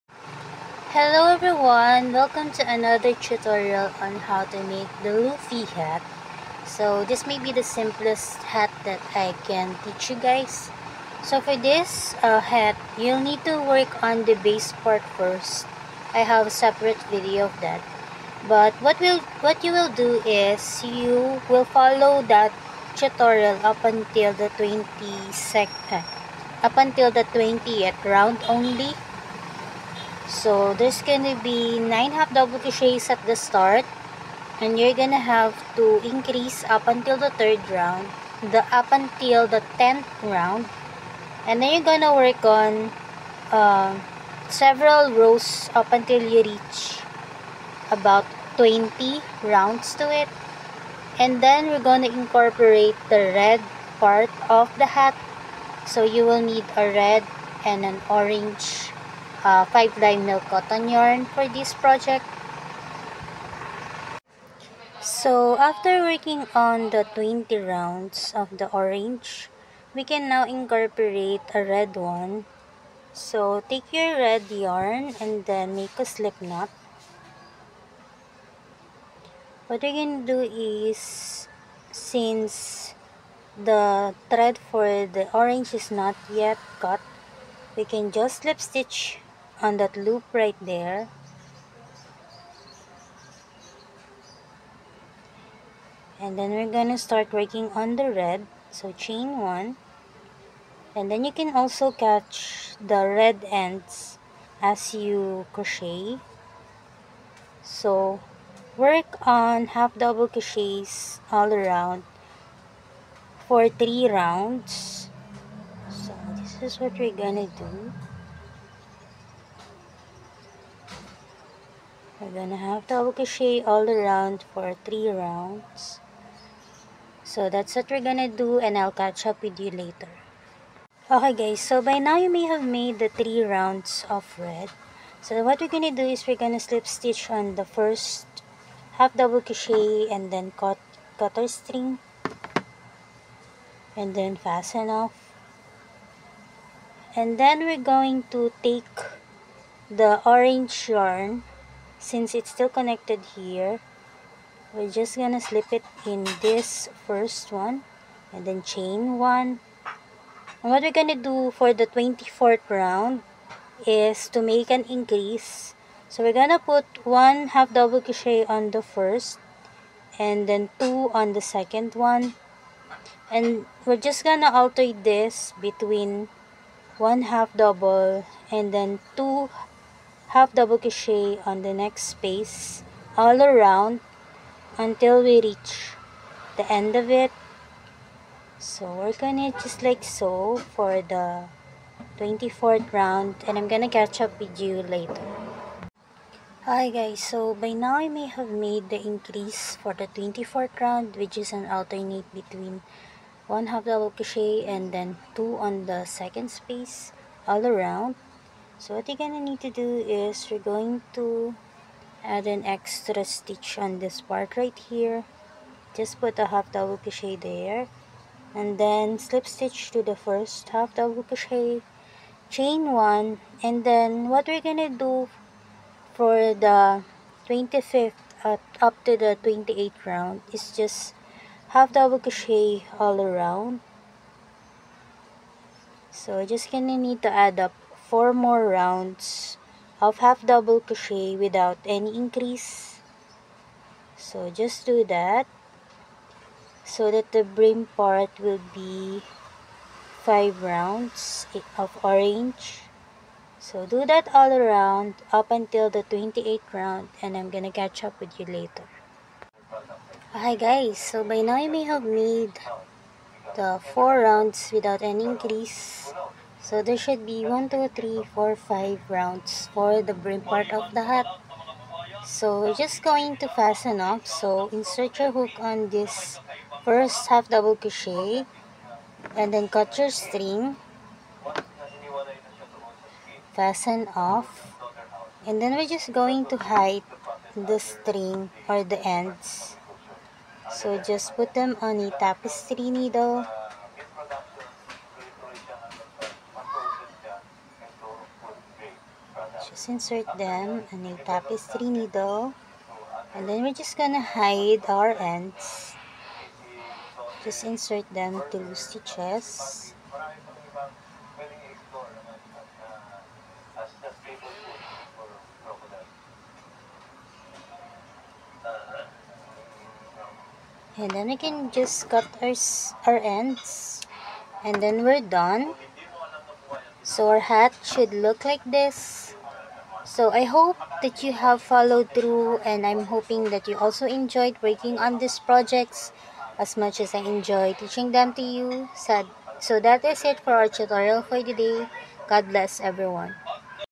How I Crochet. a Luffy Hat
How I Crochet my Character Hats: Base

Materials: Milk Cotton 5 ply Yarn. 3-4mm Hook,
Scissors
I hope you enjoyed working on this project
Playlist available for it titled 'Tutorials Start'